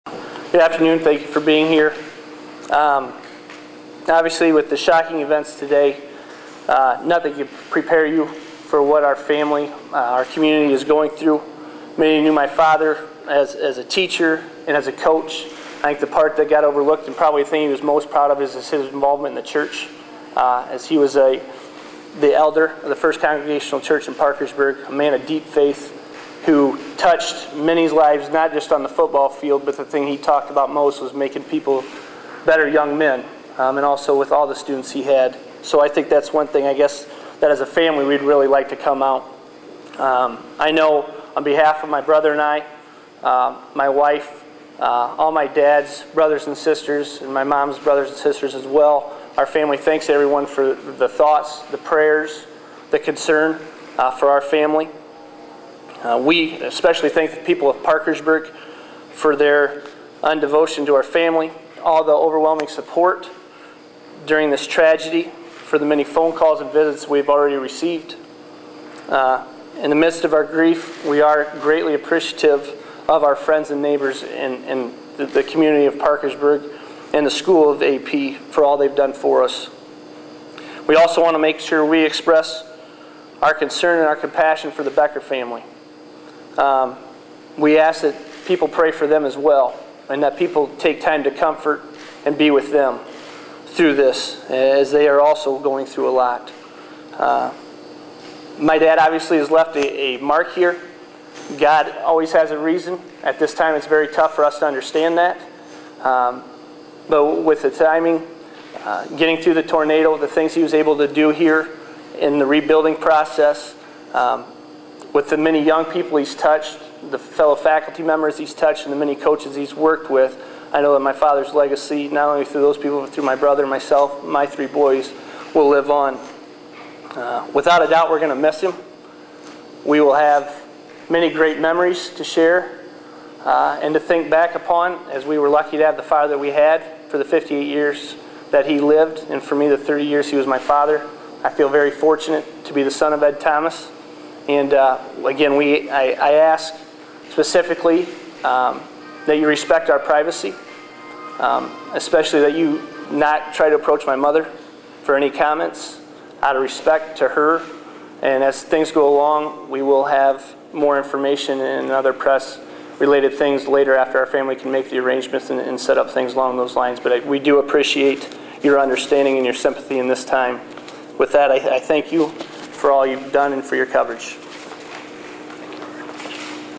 Ed Thomas' son speaks about his dad
Aaron Thomas talks about the character and legacy of his father, Ed Thomas. Ed Thomas, the legendary football coach at Aplington-Parkersburg High School in Iowa, was shot and killed Wednesday, June 24, 2009.